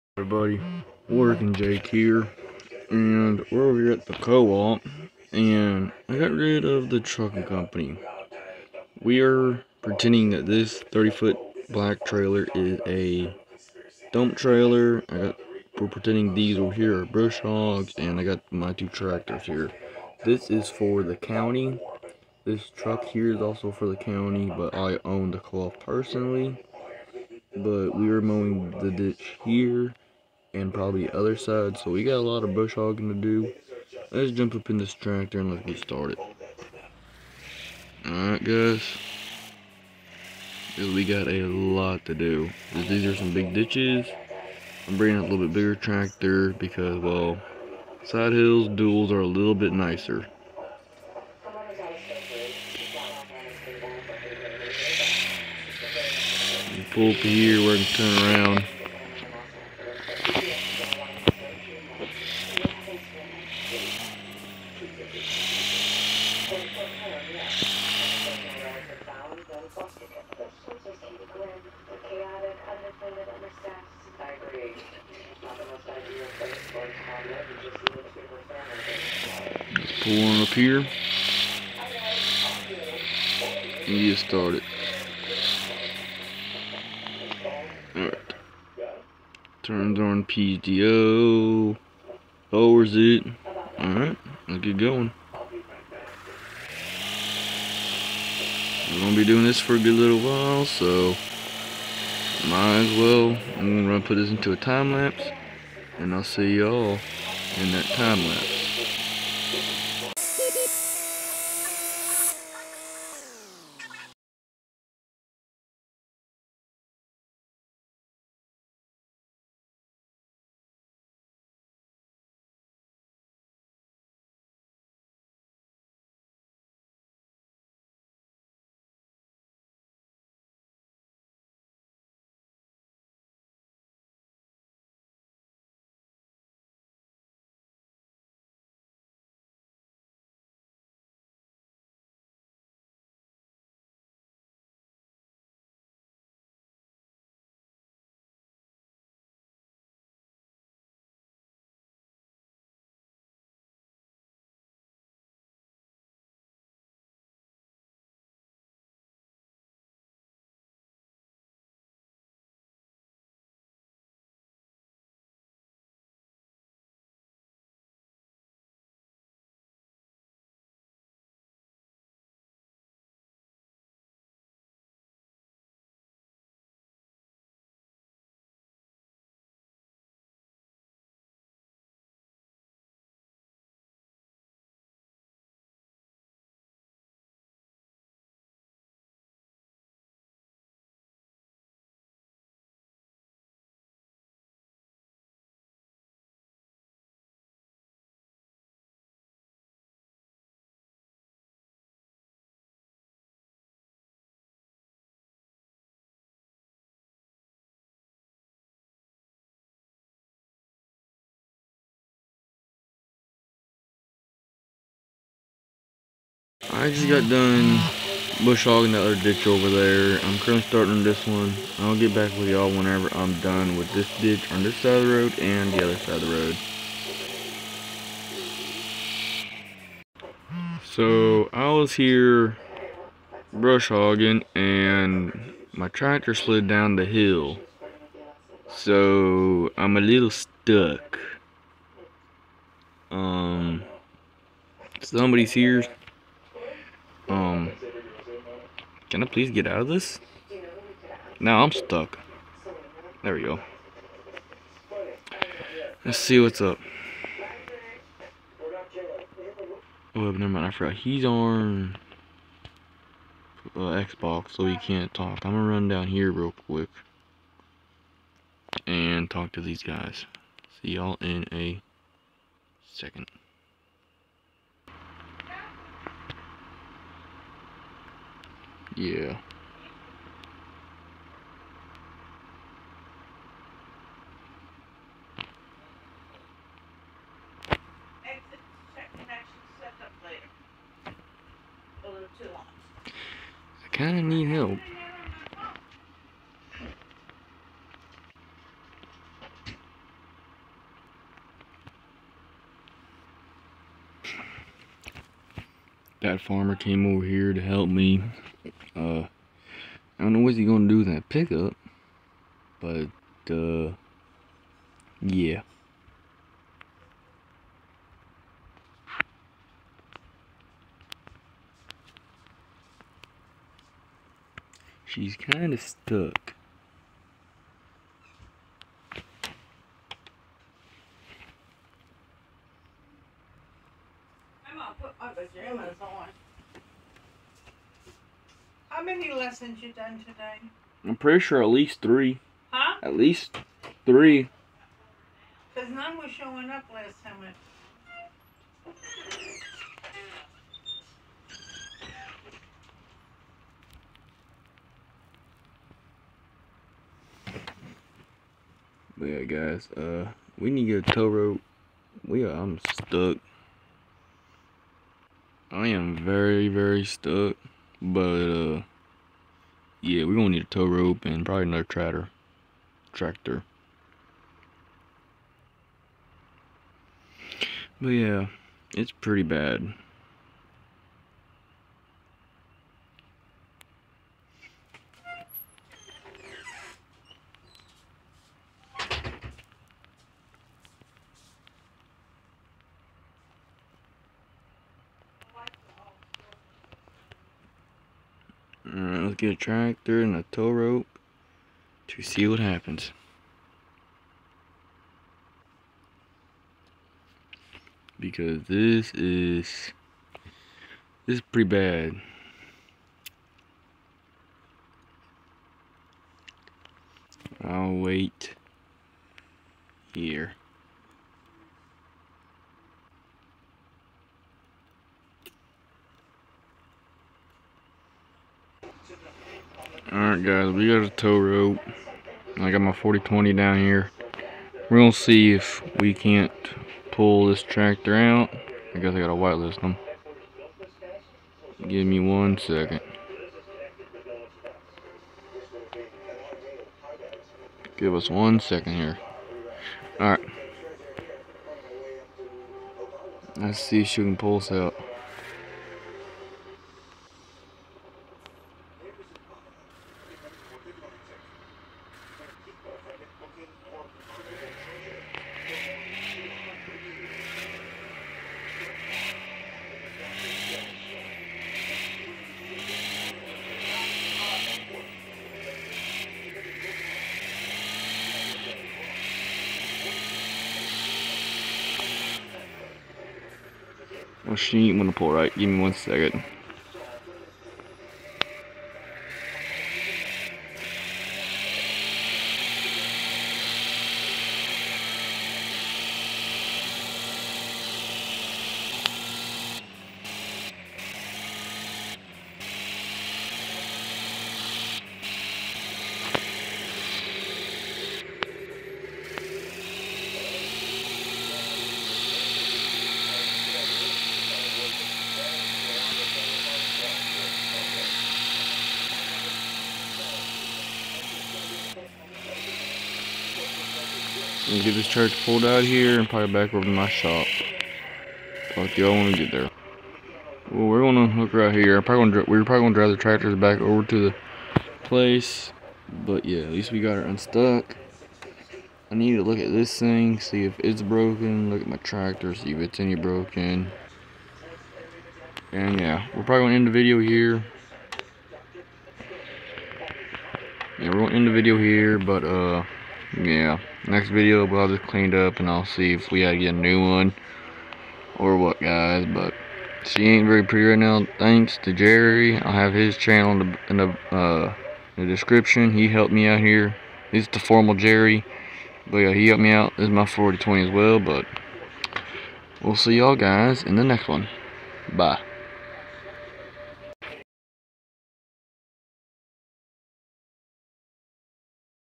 Mowing the ditches for the county
Brush hogging the ditches but the tractor slid down the hill so I got stuck and I had to ask for help to get unstuck

My buddy's channel that helped me get unstuck @formaljerry